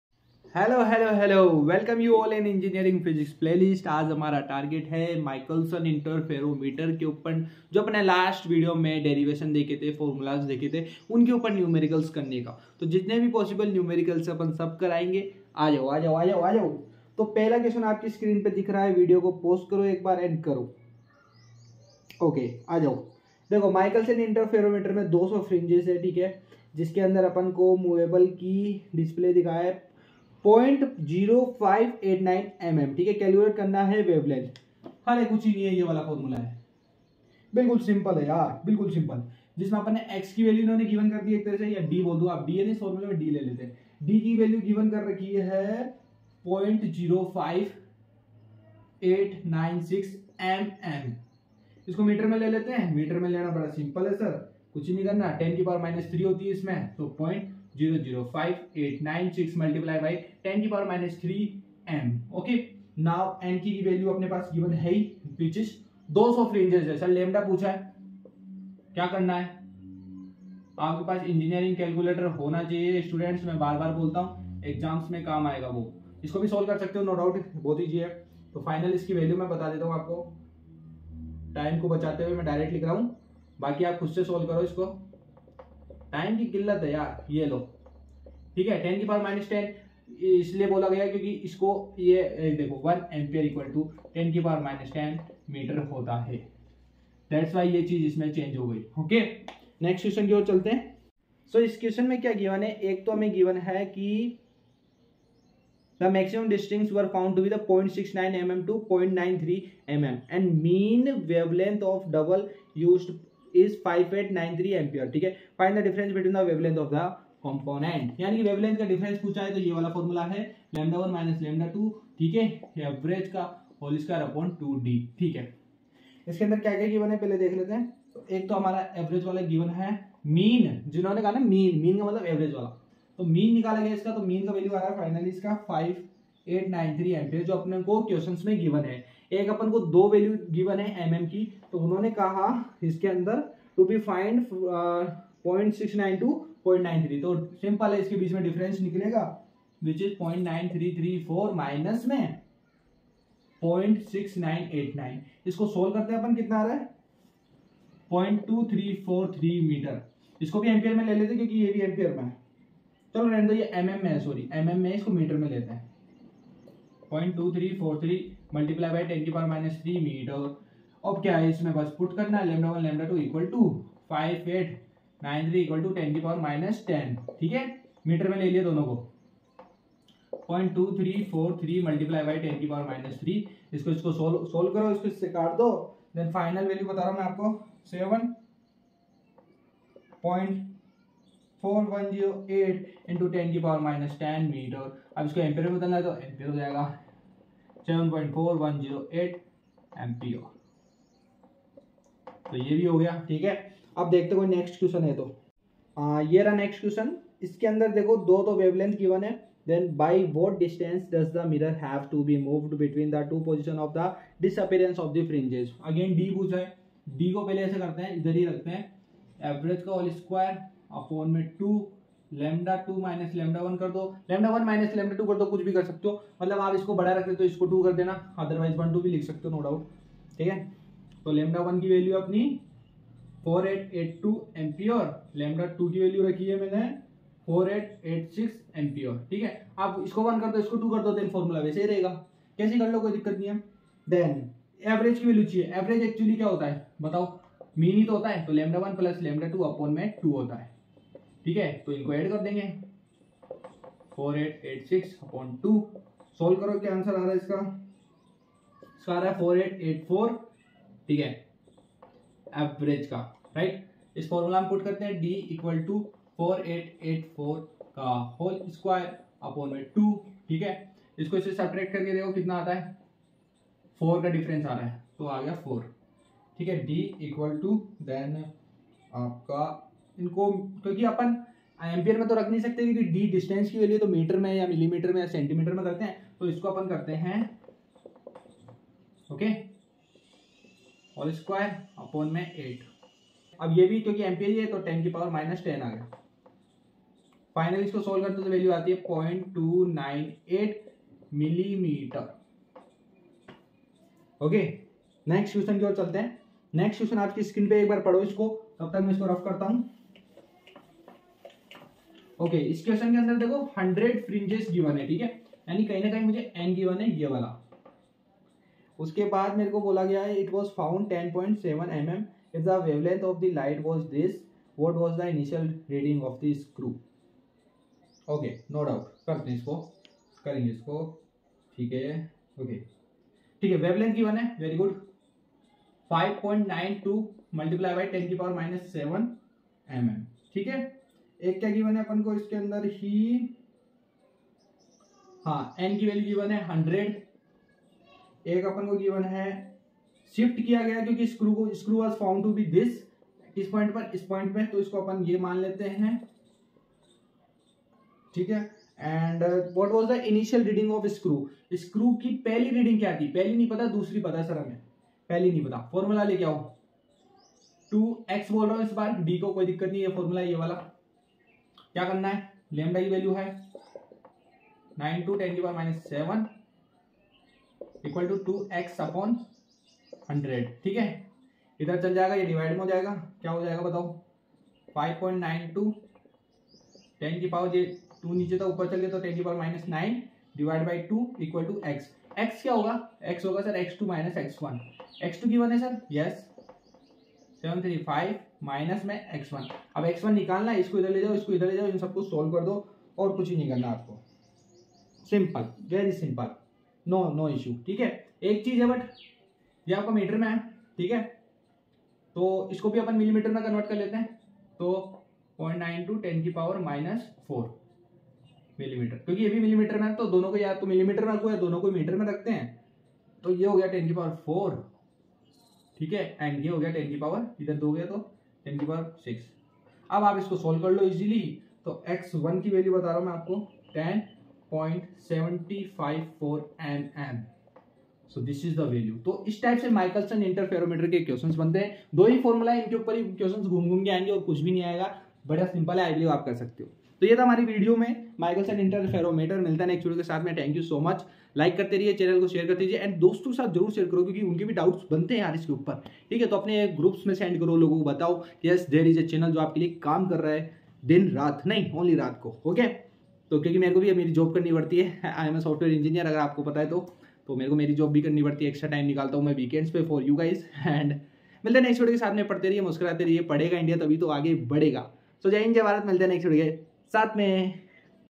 Numericals on Michelson Interferometer in engineering physics | Lec-6
In this video, We solved numericals on Michelson Interferometer